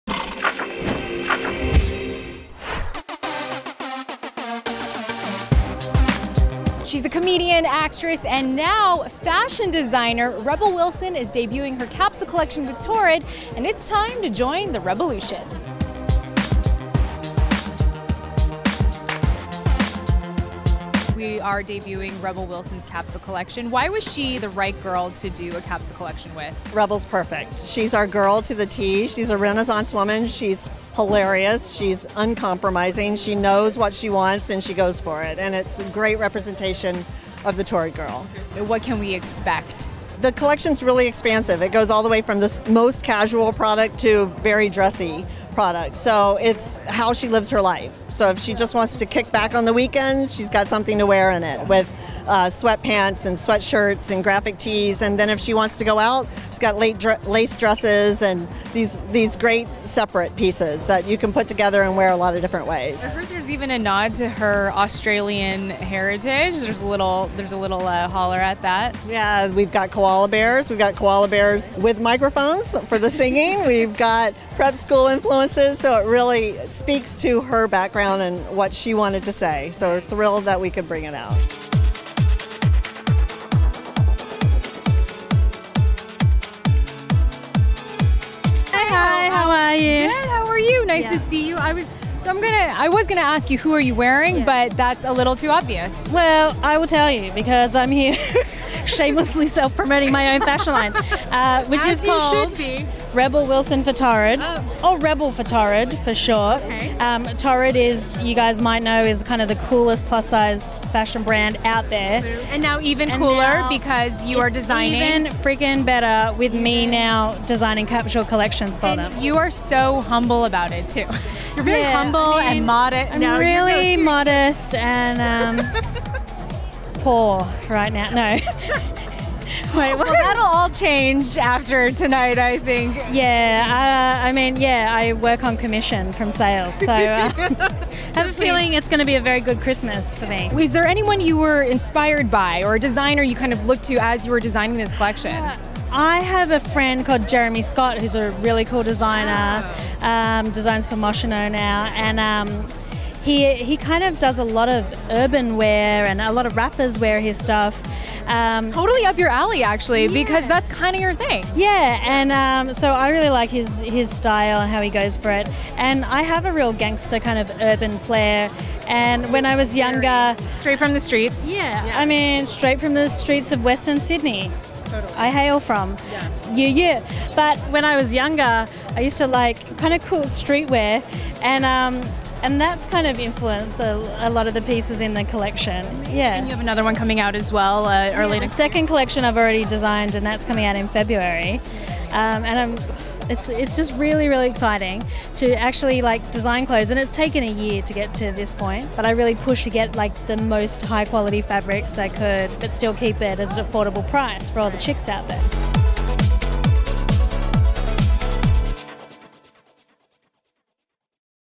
Rebel Wilson Launches Her Urban-Inspired Fashion Line!
YH is on the scene for the launch of actress Rebel Wilson's exclusive fashion line of fierce clothing and accessories for Torrid! CEO Lisa Harper tells us what Rebel brings to their brand, plus Rebel herself explains to us what and who inspired her designs!